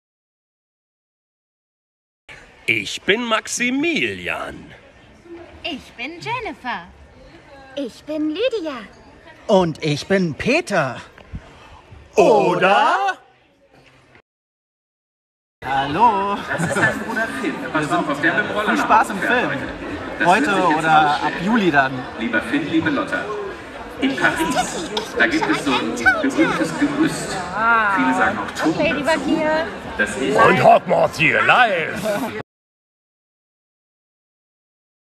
Synchronsprecher von Ladybug, Cat Noir, Tikki & Hawkmoth (Miraculous Ladybug)
Synchronsprecher von Ladybug (Lydia Morgenstern), Cat Noir (Maximilian Artajo), Tikki (Jennifer Weiß) & Hawkmoth (Peter Flechtner) bei der Kinopremiere von LADYBUG & CAT NOIR der Film